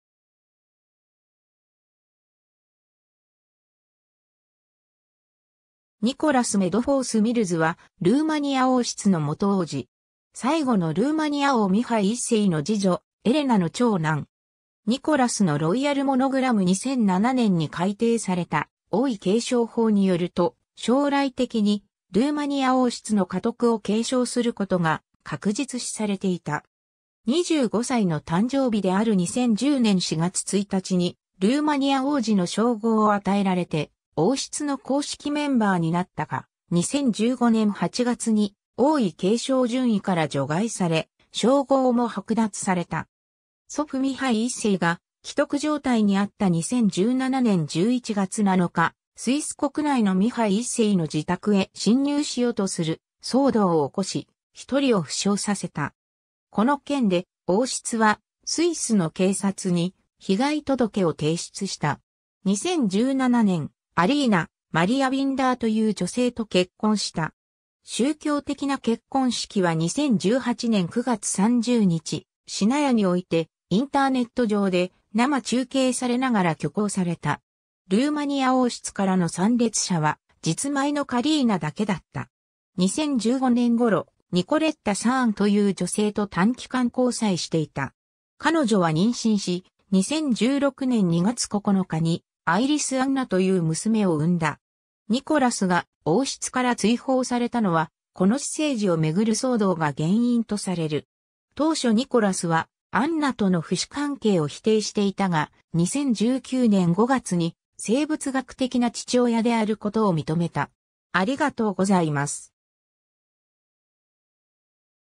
ニコラス・メドフォース＝ミルズ
ニコラス・メドフォース＝ミルズ（Nicholas Medforth-Mills）は、ルーマニア王室の元王子。
最後のルーマニア王ミハイ1世の次女エレナの長男。
 ニコラスのロイヤル・モノグラム 2007年に改訂された王位継承法によると、将来的にルーマニア王室の家督を継承することが確実視されていた。
25歳の誕生日である2010年4月1日に「ルーマニア王子」の称号を与えられて王室の公式メンバーになったが、2015年8月に王位継承順位から除外され、称号も剥奪された。
 祖父ミハイ1世が危篤状態にあった2017年11月7日、スイス国内のミハイ1世の自宅へ侵入しようとする騒動を起こし、一人を負傷させた。
この件で王室はスイスの警察に被害届を提出した。
 2017年、Alina-Maria Binderという女性と結婚した。
宗教的な結婚式は2018年9月30日、シナヤにおいてインターネット上で生中継されながら挙行された。
ルーマニア王室からの参列者は、実妹のカリーナだけだった。
 2015年頃、Nicoleta Cirjanという女性と短期間交際していた。
彼女は妊娠し、2016年2月9日にアイリス・アンナ（Iris Anna）という娘を生んだ。
ニコラスが王室から追放されたのは、この私生児をめぐる騒動が原因とされる。
当初ニコラスはアンナとの父子関係を否定していたが、2019年5月に生物学的な父親であることを認めた。